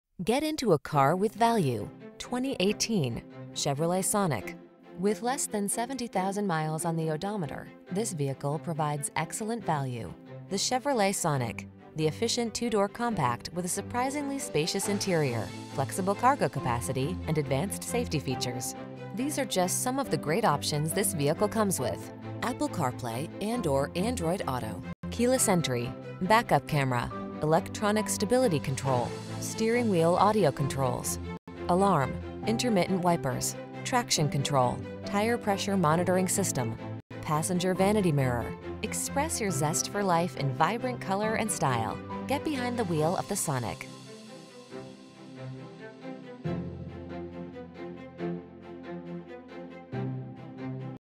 2018 Chevrolet Sonic LS Miami, Miami Beach, Homestead, Kendall, Coral Gables FL
Pre-Owned 2018 Chevrolet Sonic LS available at Ferco Motors located in 1740 SW 1st Street, Miami, FL, 33135. Servicing the Miami, Miami Beach, Homestead, Kendall, Coral Gables area.

Get into a car with value, the   2018  Chevrolet Sonic.  The Chevrolet Sonic: The efficient two-door compact with a surprisingly spacious interior, flexible cargo capacity, and advanced safety features.
 These are just some of the great options this vehicle comes with: Apple Carplay®/Android Auto®, Keyless Entry, Back-Up Camera, Electronic Stability Control, Steering Wheel Audio Controls, Alarm, Intermittent Wipers, Traction Control, Tire Pressure Monitoring System, Passenger Vanity Mirror. Express your zest for life in vibrant color and style. Get behind the wheel of the Sonic.